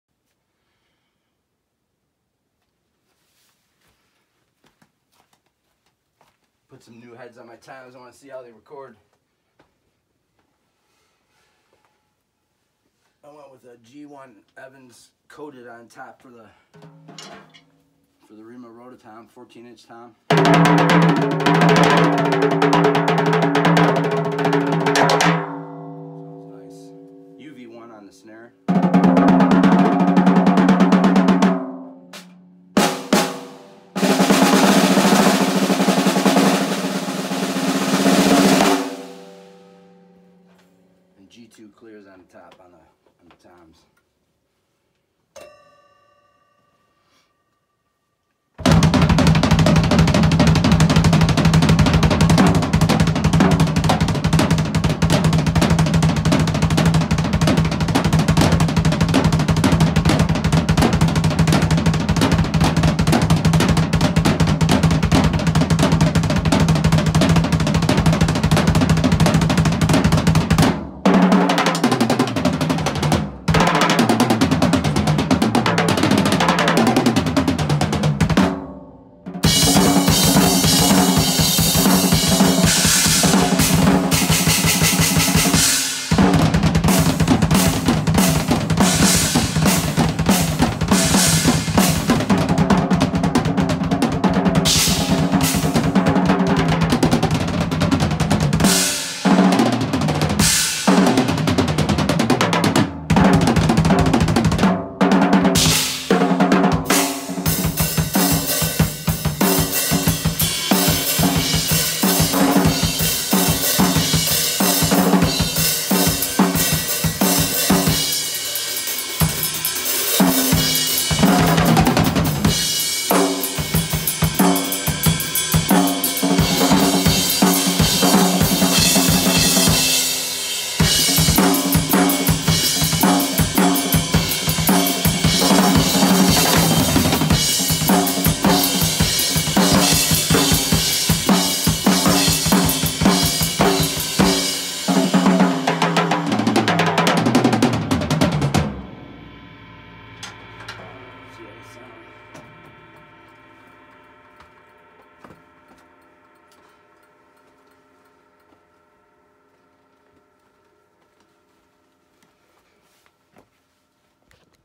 Rob A Drums
I changed my heads to Evans G2 clear on my toms, (I have always used coated) and I switched my clear head to a G1coated on my 14" roto tom and UV1 on my snare, just giving them a test run, I love them!